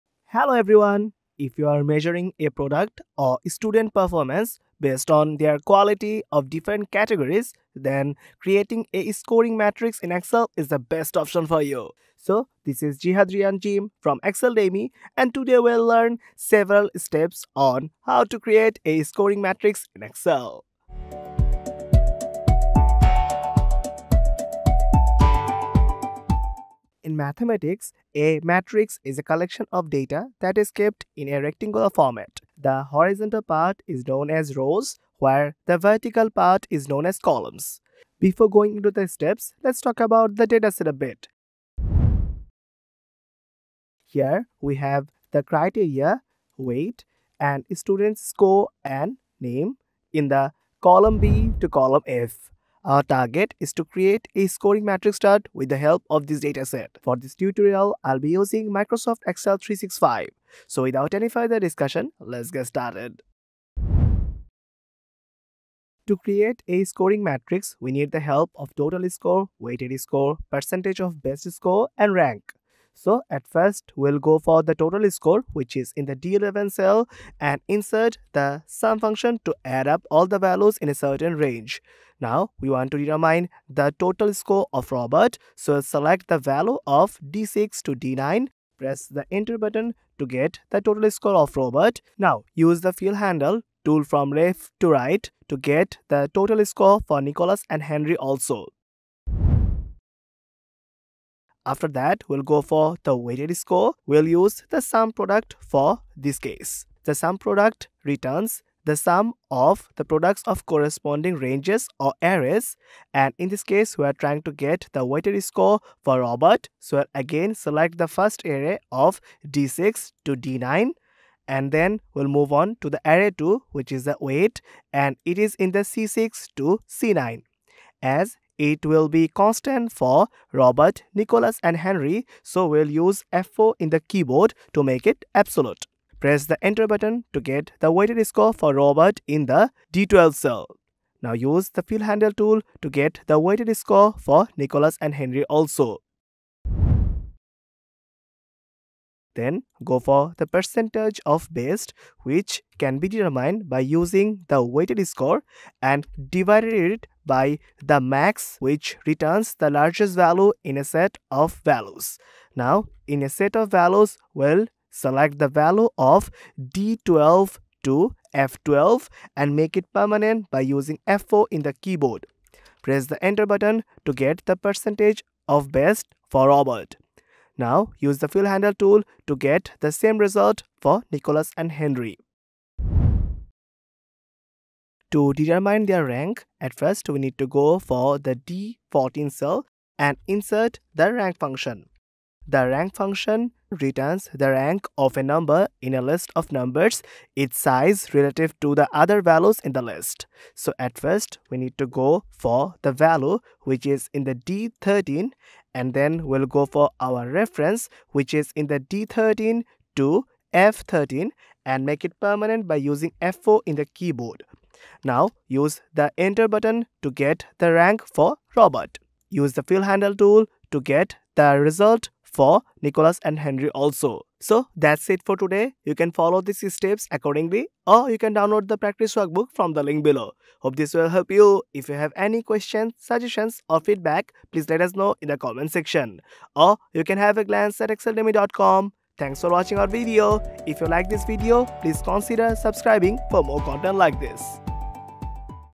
How to Create a Scoring Matrix in Excel
In this video, I'll guide you through multiple steps to create a scoring matrix in Excel.  You'll learn about making criteria first, calculating the total score, finding a weighted score, calculating the percentage of the best, and determining rank. A scoring matrix can be used to analyze databases and evaluate the performance of employees, students, or other individuals. With practical examples and step-by-step instructions, you can effortlessly make a scoring matrix in your Excel spreadsheets.

👨‍🏫 Instructor: Zehad Rian Jim
🎥 Editor: Md. Riajul Islam

✨ ⯆ Resources:
Alt + = - To apply the SUM function

0:00 - Intro
0:44 - Making criteria first
1:05 - Calculating total score
1:42 - Finding a weighted score
2:29 - Calculating the percentage of the best
2:29 - Determining rank